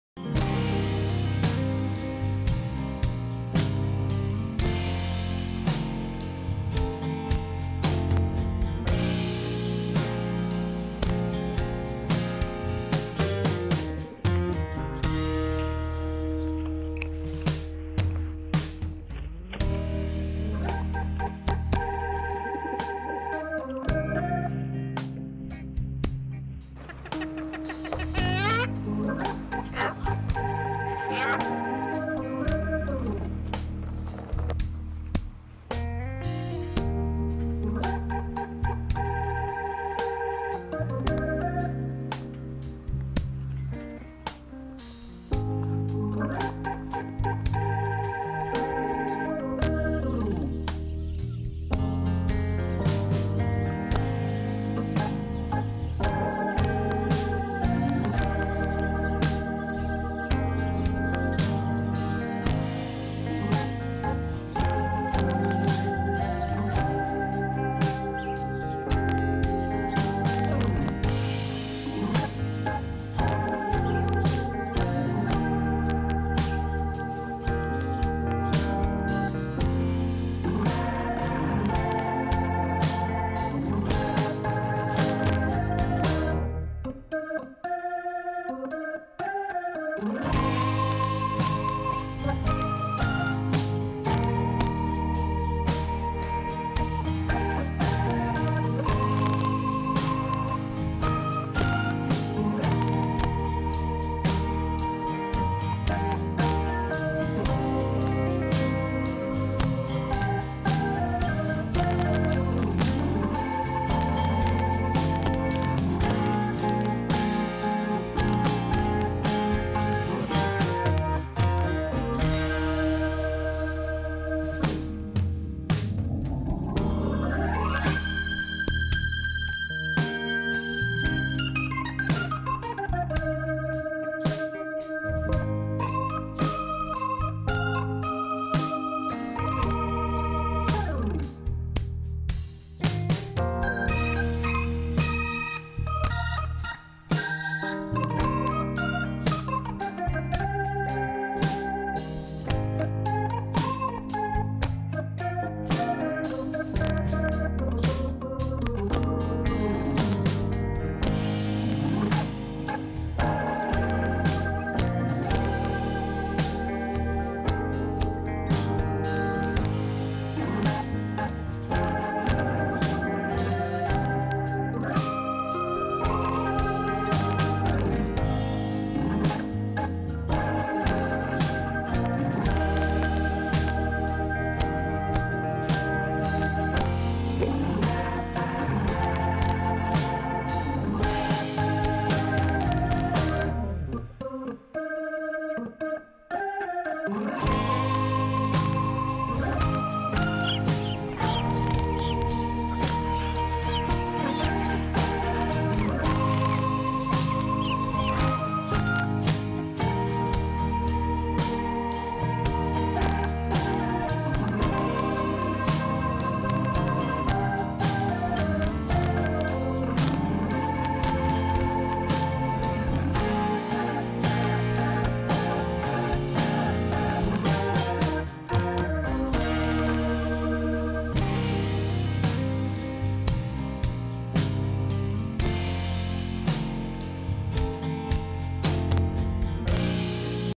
Unlikely Animal Friendships Unbelievable Animals Friends Animal Friendship Videos
Can animals be best friends? Unbelievable, unlikely, amazing animal friendships between different species that you won’t believe! Tiger and baby goat,
antelope and kitten, cockatoo and dog and so much more in this unbelievable, unlikely animal friendships video.